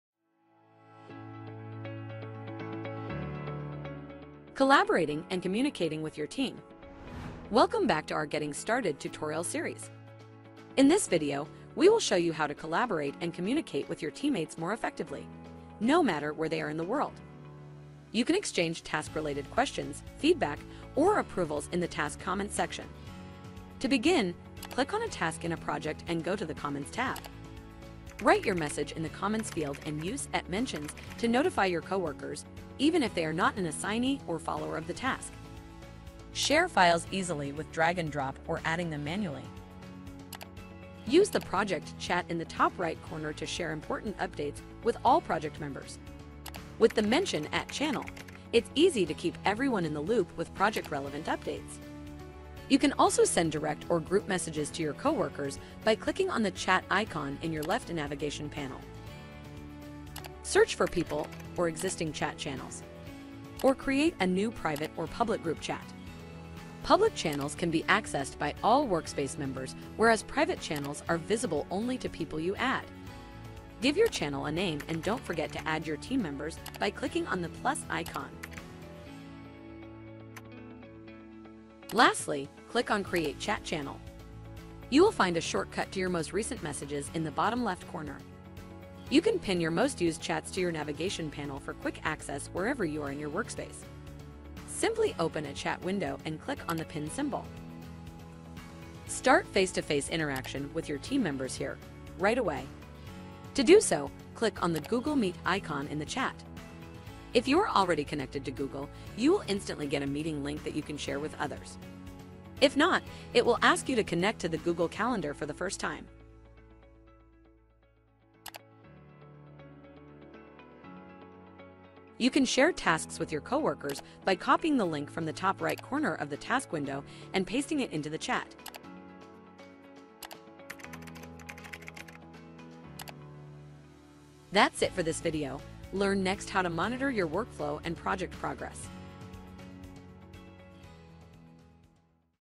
Getting started with Taskworld - Part 4: Collaborating and Communicating with your Team
Learn about how to collaborate and communicate with teams and individuals on Taskworld.